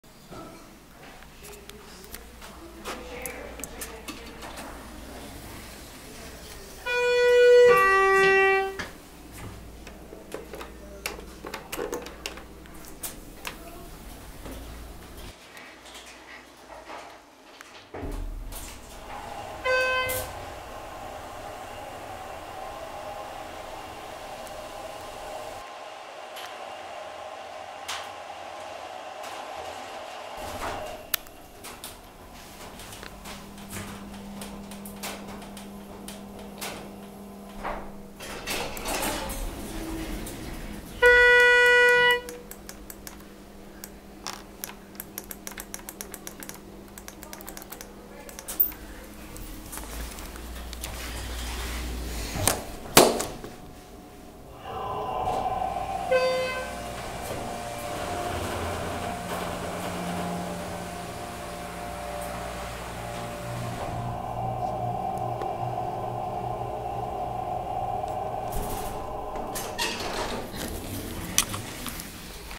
Another elevator at horse museum
This is the second elevator at the Horse Museum, which is a Schindler.